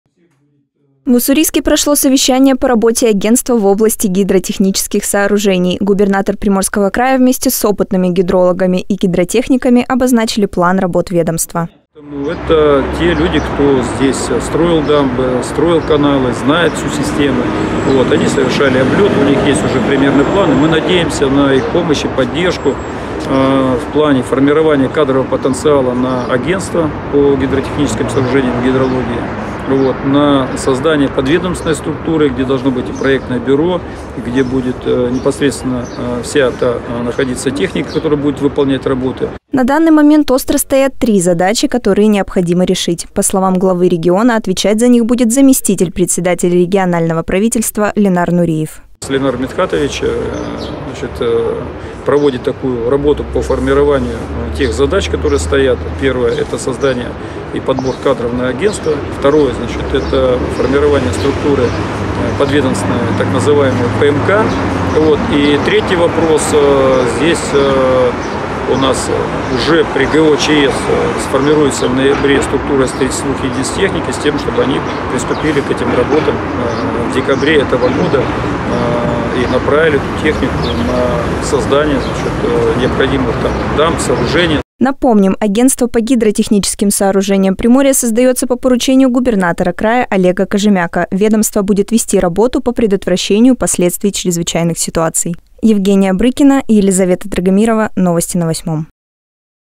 Олег Кожемяко обсудил с экспертами работу создаваемого ведомства по гидрологии.
В Уссурийске прошло совещание по работе агентства в области гидротехнических сооружений. Губернатор Приморского края вместе с опытными гидрологами и гидротехниками обозначали план работ ведомства.